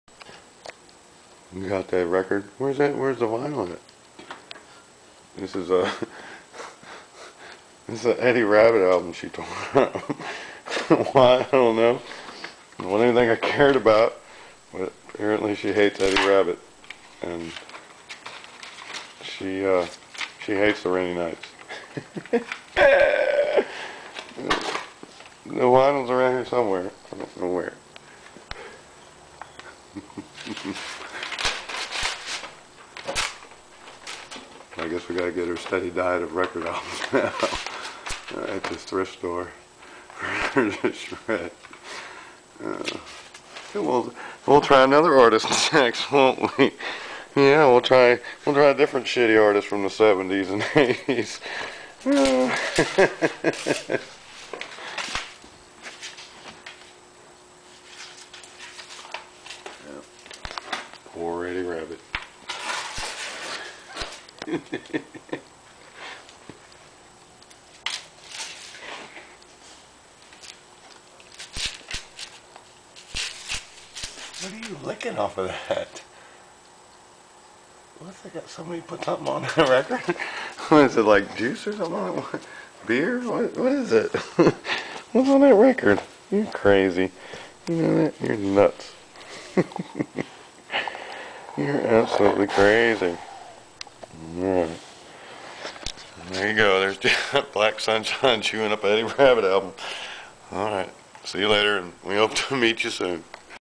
Jet Black Sunshine - Black German Shepherd Destroying an Eddie Rabbit Vinyl Album...
My Beautiful Puppy Dog, Jet Black Sunshine - a Black German Shepherd/Crazy Mix that for some reason, hates Eddie Rabbit...  When Jet Black Sunshine gets agitated, she goes in the back of my warehouse and shreds Eddie some more...